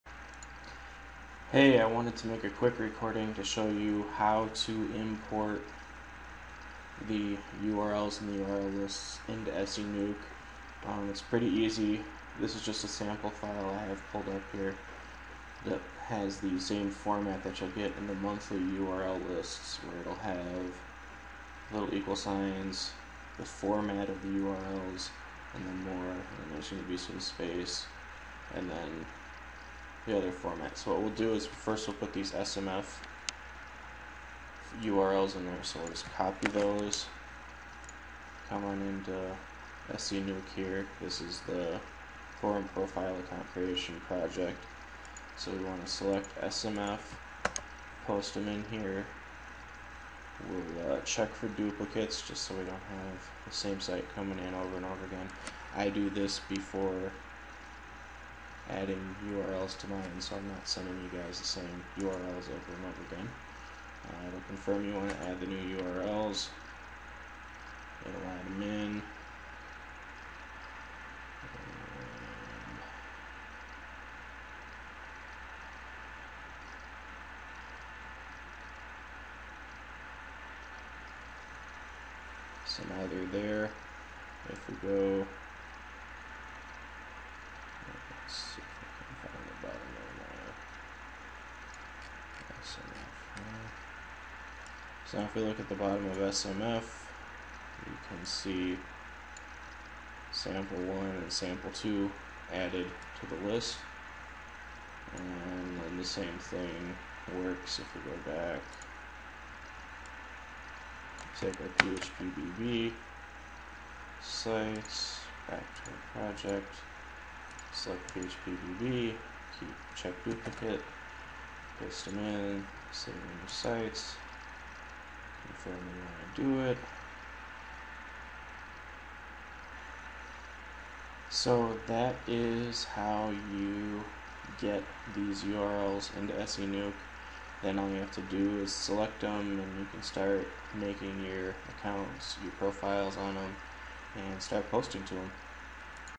Adding the Monthly Profile URLs to SEnuke
This video shows you how to add your monthly forum profile URLs to SEnuke